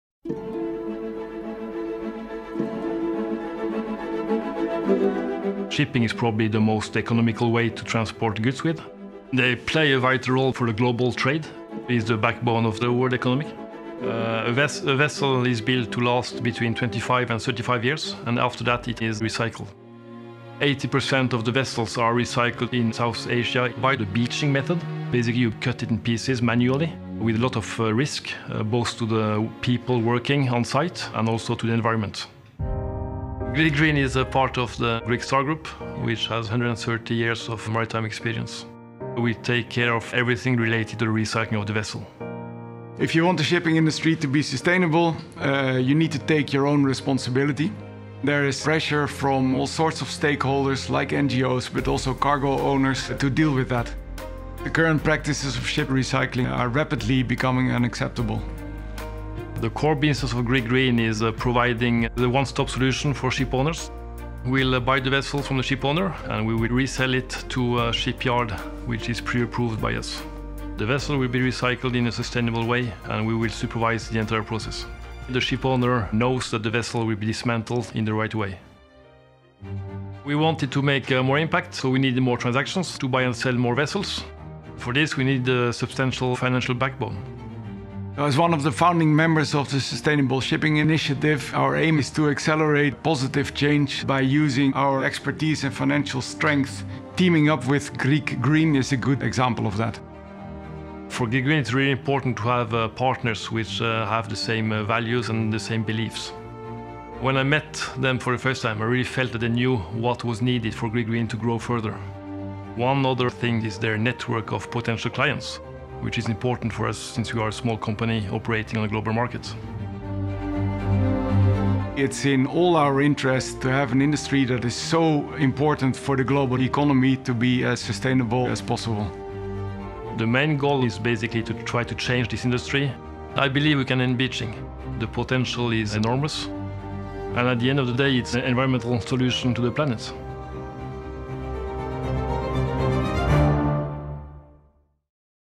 Grieg Green - Enabling sustainable ship recycling
Grieg Green offers ship owners an alternative to the controversial “beaching” method to demolish ships. Instead of dismantling ships on beaches causing long term adverse effects to the environment as well as health risks to workers, Grieg Green offers an advanced sustainable recycling solution. Watch how Grieg Green is able to make more impact and bring positive change to the shipping industry.